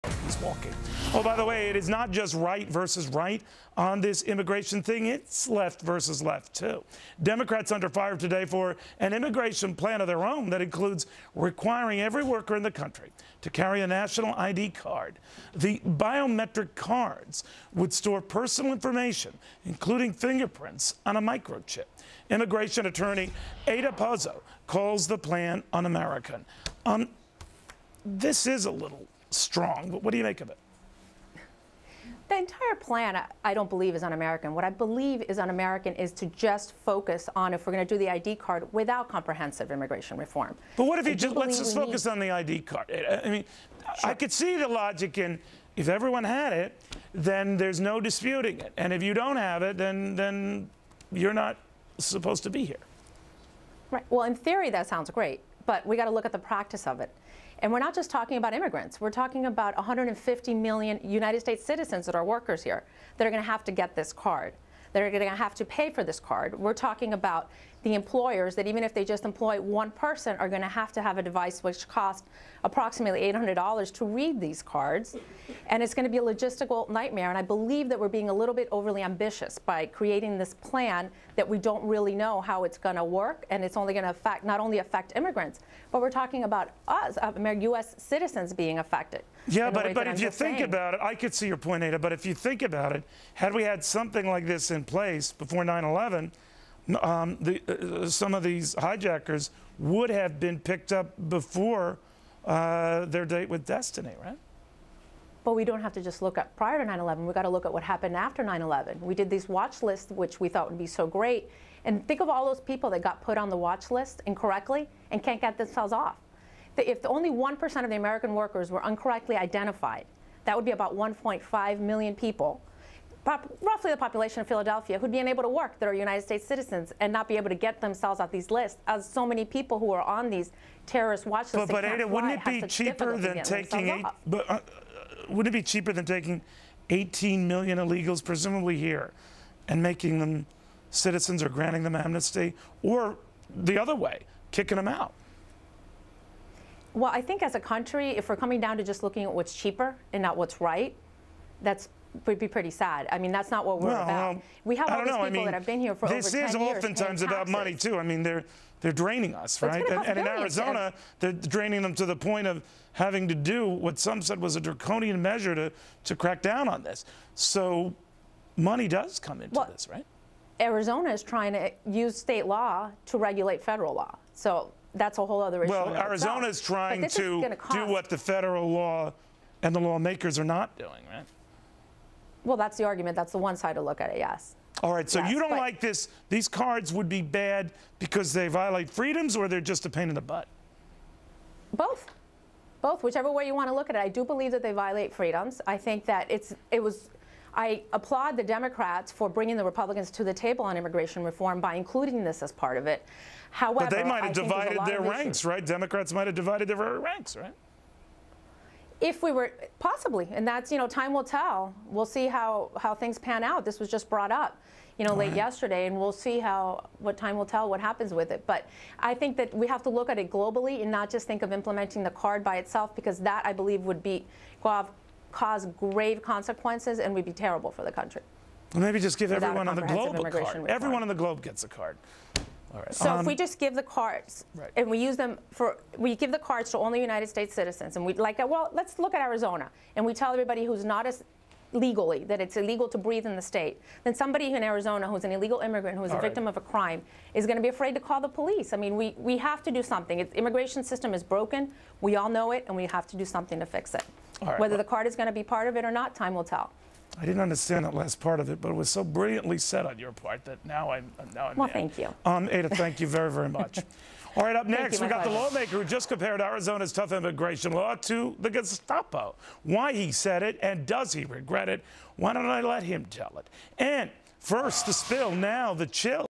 Immigration Attorney - Ada Pozo on Fox News
Fox News discusses the Democratic immigration plan on America, immigration lawyer Ada B. Pozo gives her opinion regarding immigration cards.